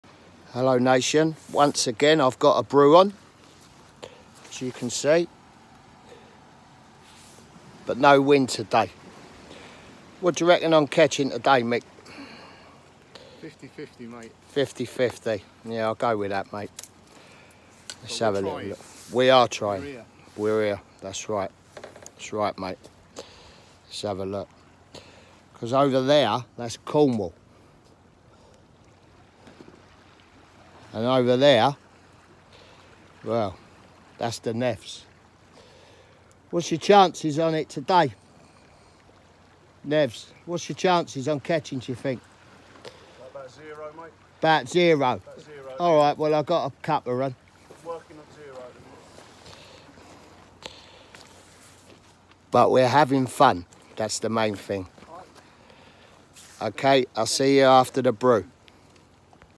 HELLO NATION: Part 2 - Fishing at Devil's Point, Plymouth (14/10/19). "We are trying hard..."
Another brew... not happy campers today. Wish we could catch something for you, Nation. We are trying... See you after me brew.

Hello Nation is my amateur parody fishing, camping and general outdoor reviewing account.